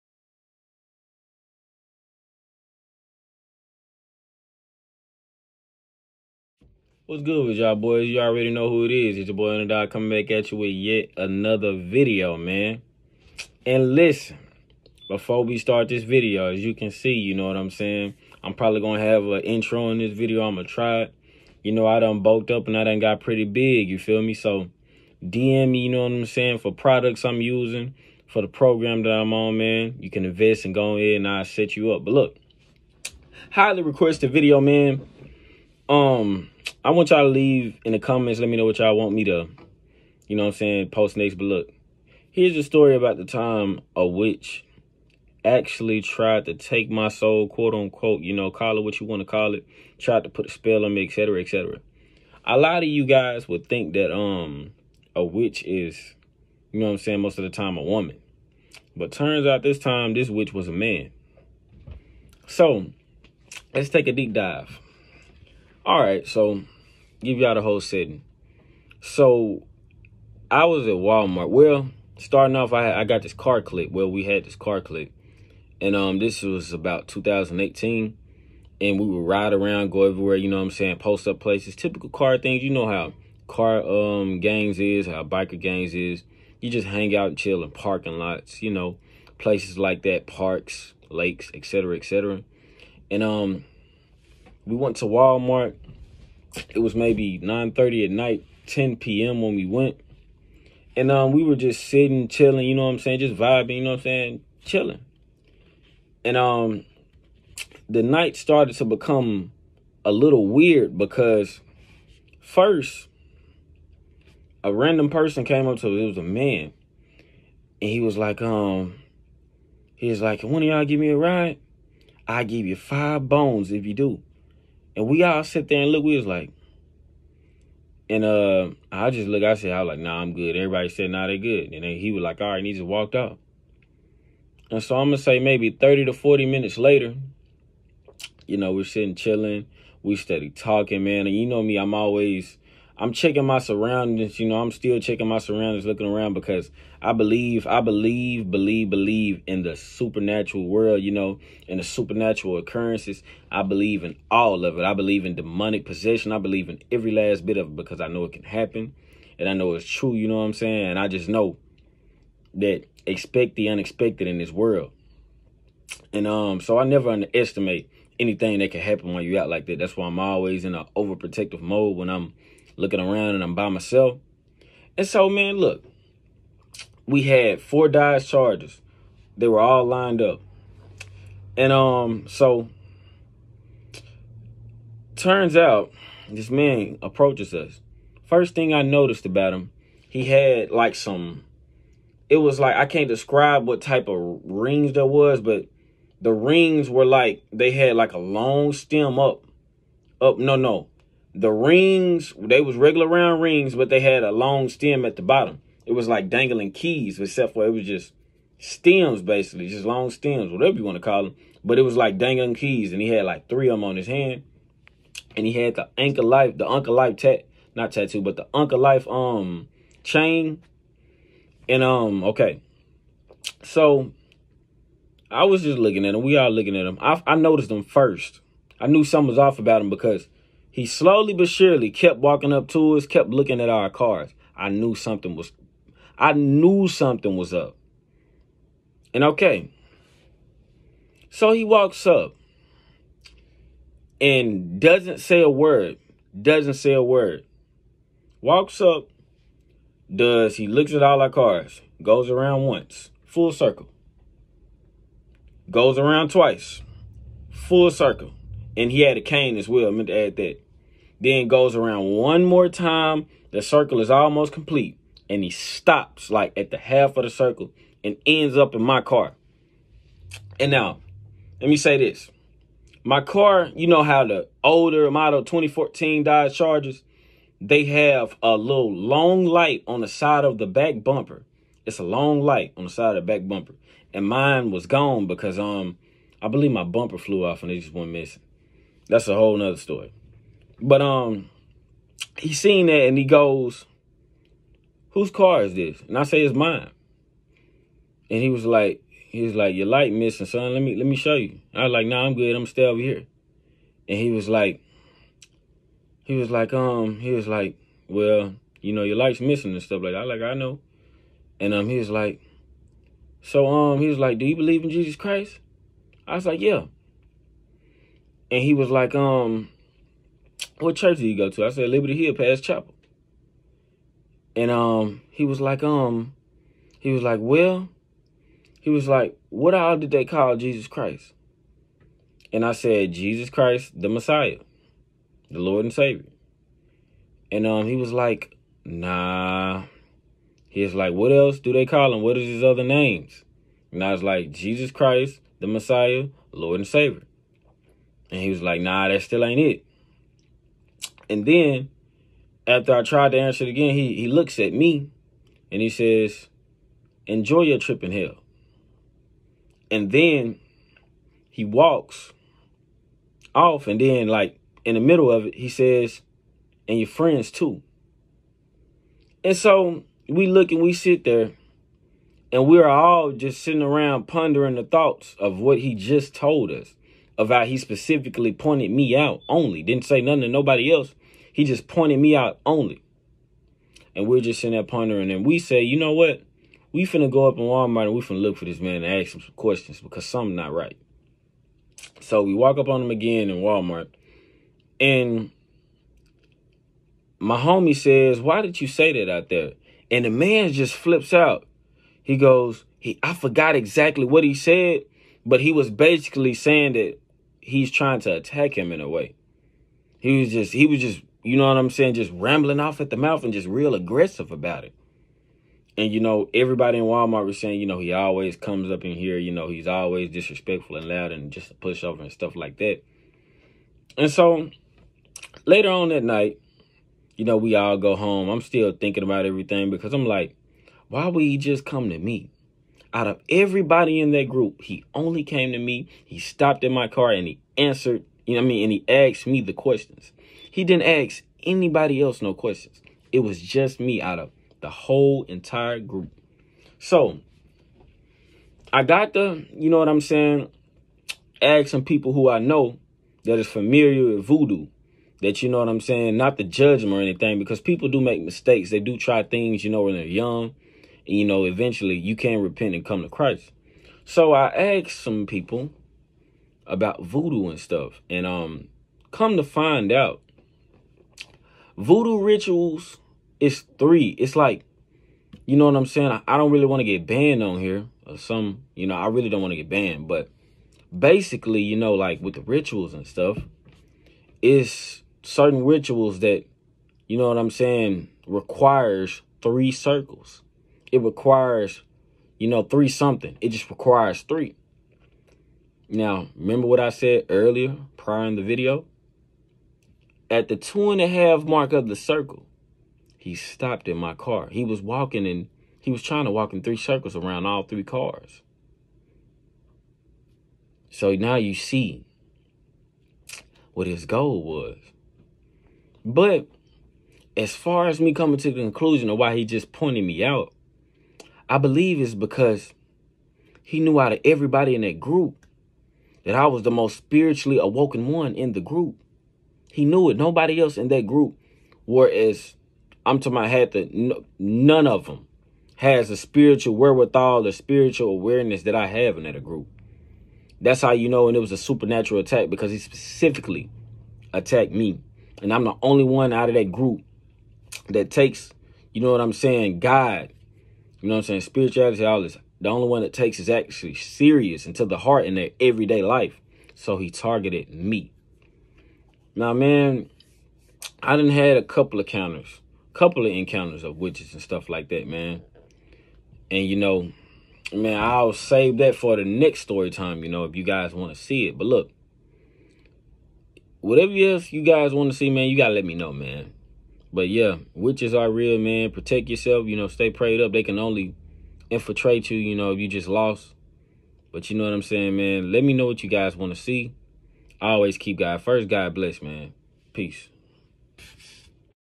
A WITCH TRIED TO PUT A SPELL ON ME (Story Time)
For more info about getting in the best shape
Dm here@
Facebook Kalip Watson

Let me know what else you guys wanna see